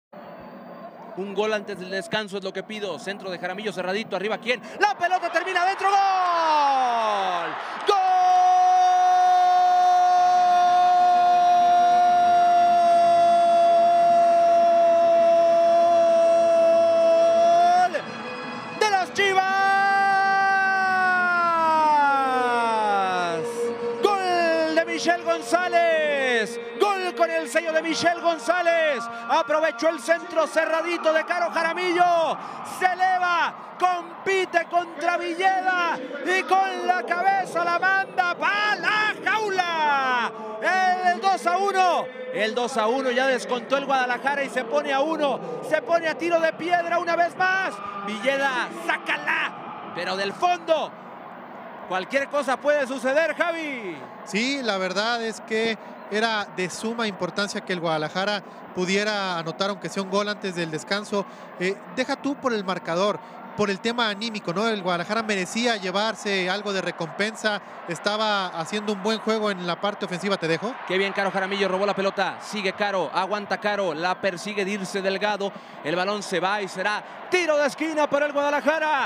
¡Gol de Michelle González! | Chivas Femenil 1-2 Pumas | Cuartos de Final Vuelta | Clausura 2022
¡Michelle González acortó distancias en el marcador con este cabezazo dentro del área!